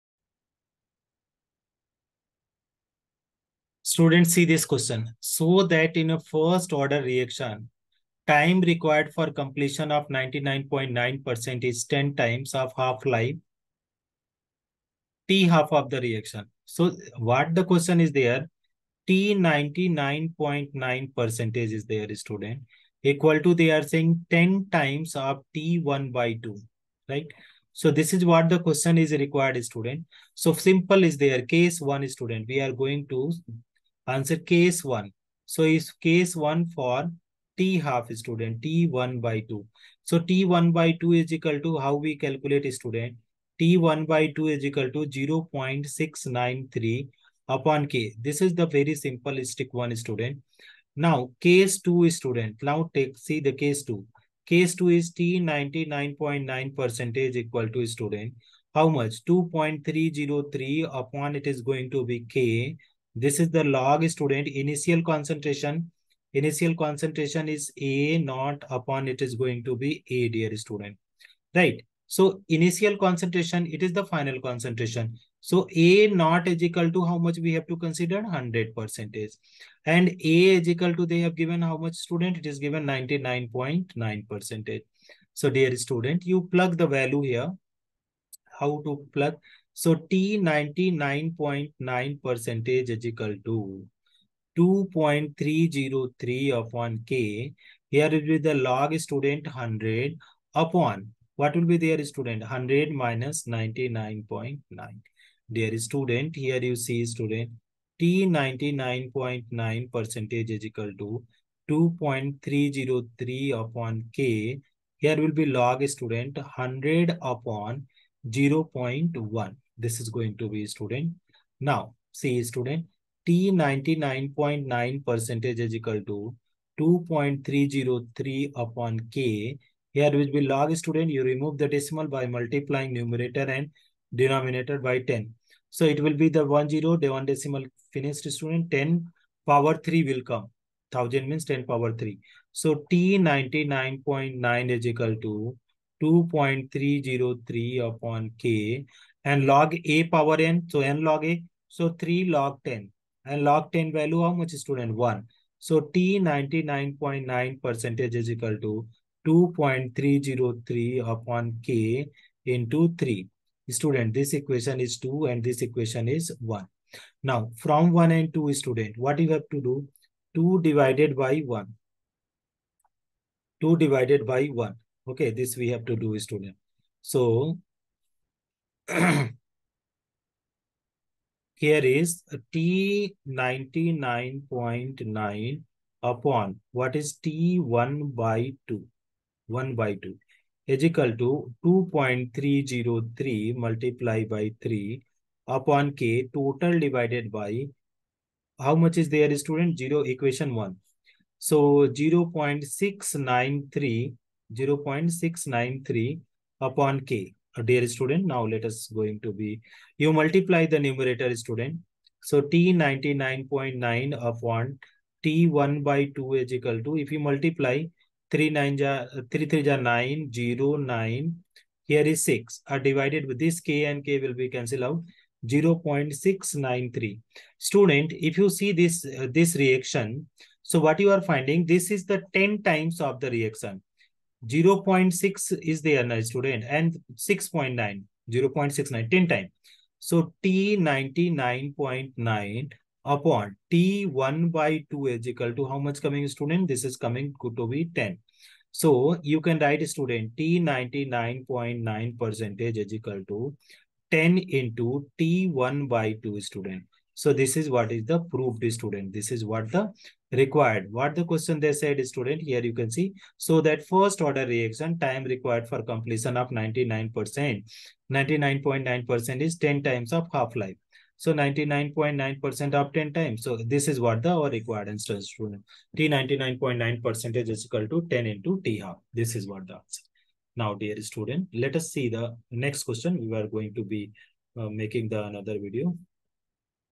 Show that in a first order reaction, time required for completion of 99.9% is 10 times of half-life
Show that in a first order reaction, time required for completion of 99.9% is 10 times of half-life (t1/2) of the reaction.